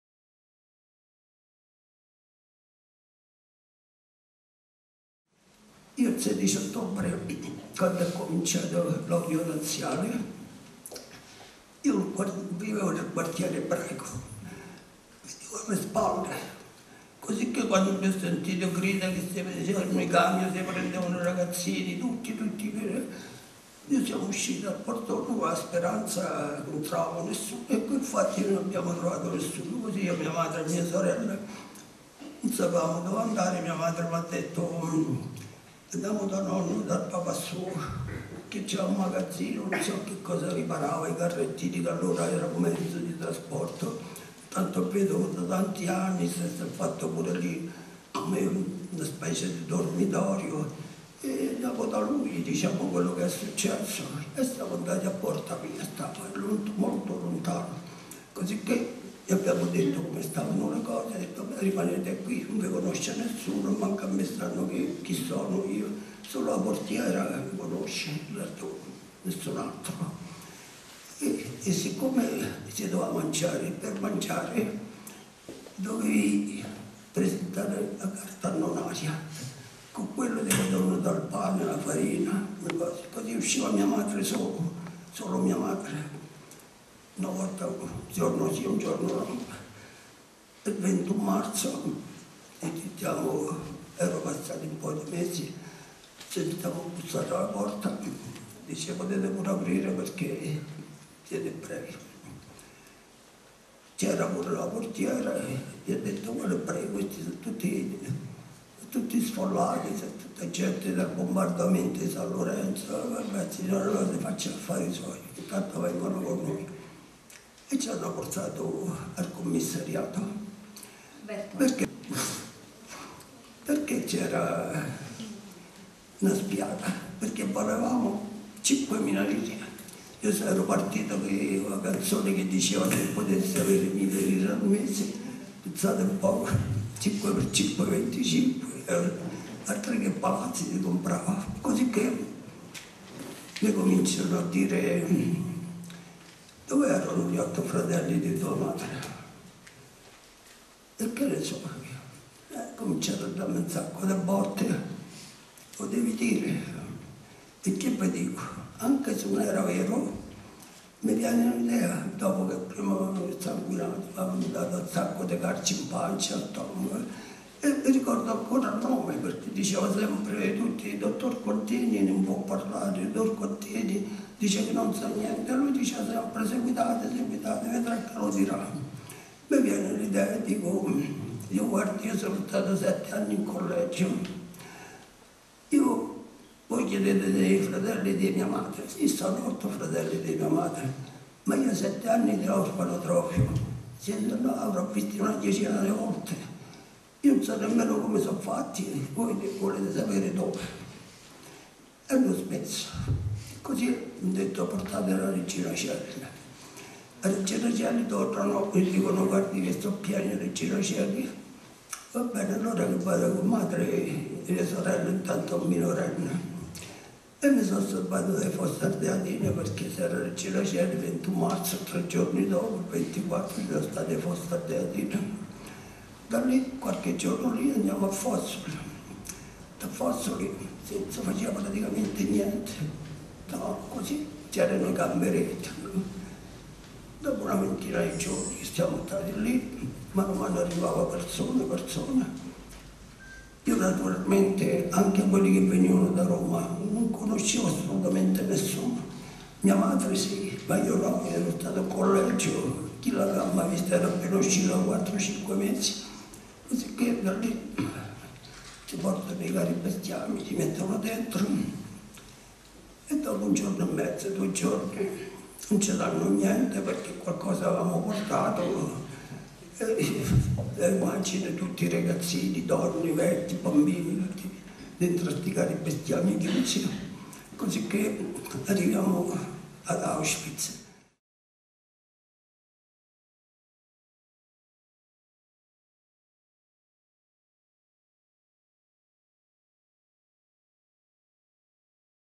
Alberto Sed 1 La partenza
Il 16 ottobre Alberto Sed aveva 15 anni e viveva al Ghetto. Quando ha sentito le grida, è uscito nella speranza di non trovare nessuno. Sono riusciti a scappare e sono andati dal nonno a Porta Pia che riparava carrettini. Nessuno li conosceva ma per mangiare bisognava presentare la tessera annonaria. Il 21 marzo, erano passati cinque mesi, bussarono. La portiera cercò d dire che erano sfollati e non ebrei ma c'era stata una spiata e qualcuno li aveva venduti. Ad Alberto, proprio perché un ebreo valeva 5mila lire, gli chiesero degli otto fratelli della madre. Alberto non rispondeva e cominciarono a picchiarlo. Alla fine l'ordina fu d portarli a Regina Coeli ma il carcere era pieno e li mandarono direttamente a Fossoli. Per fortuna, perché tre giorni dopo i prigionieri di Regina Coeli sono finiti alle Fosse Ardeatine. Da Fossoli, dopo un paio di settimane, sono stato portati nei carri bestiame ad Auschwitz